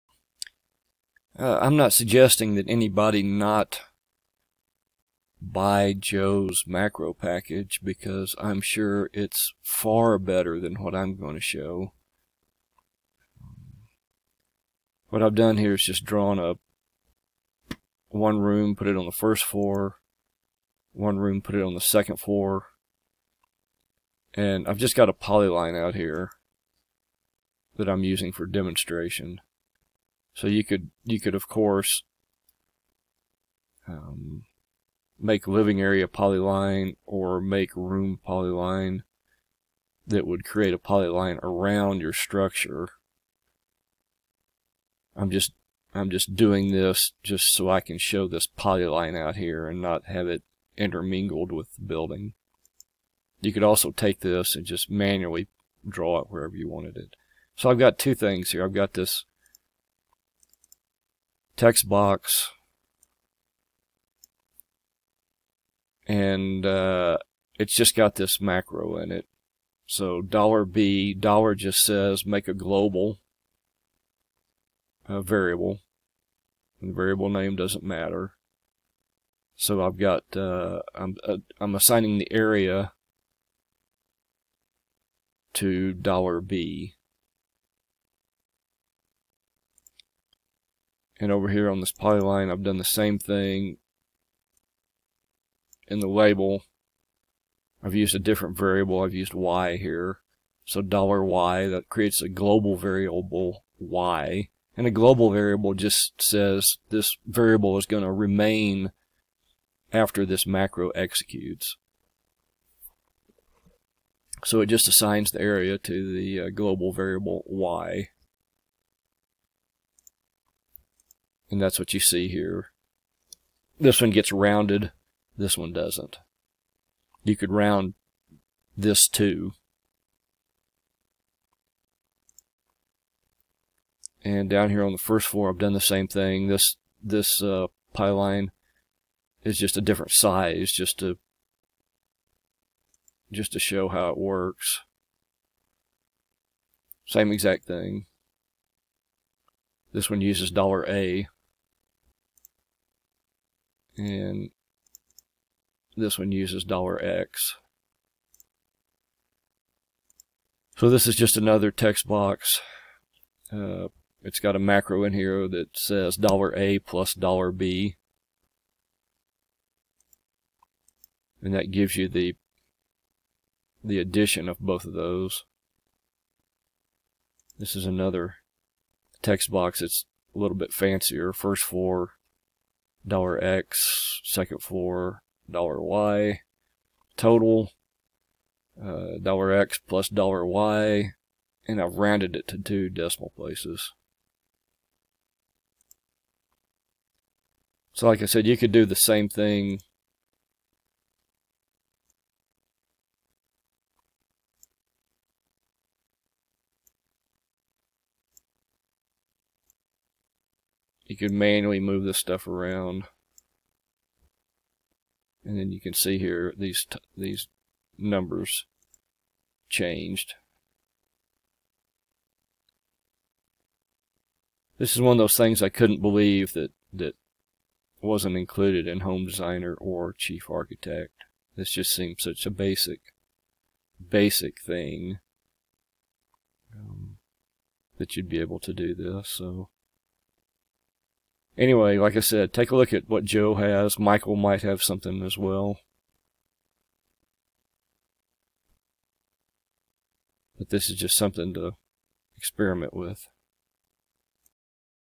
Totaling Living Area - Multiple Floors - Chief Architect
Using Polylines and a couple of macros to total areas from multiple polylines.